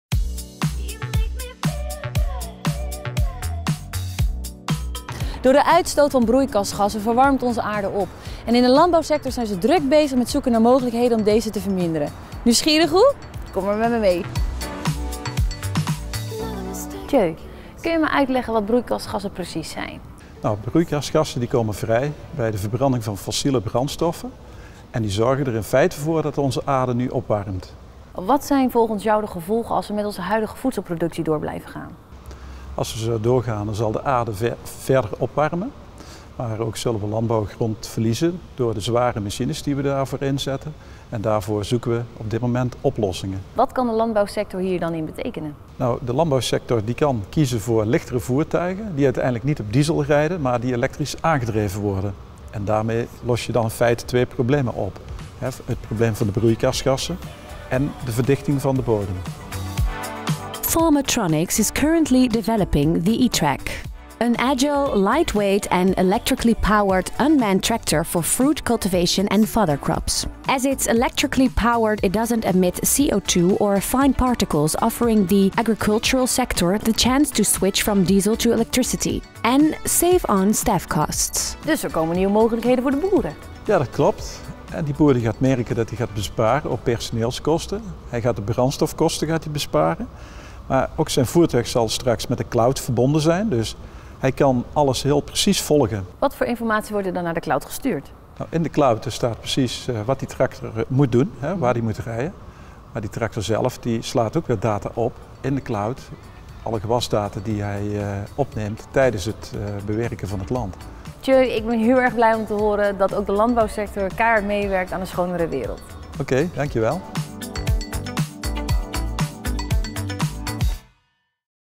Farmertronics eTrac-20
The eTrac-20 is an agile light weight robot tractor for fruit cultivation.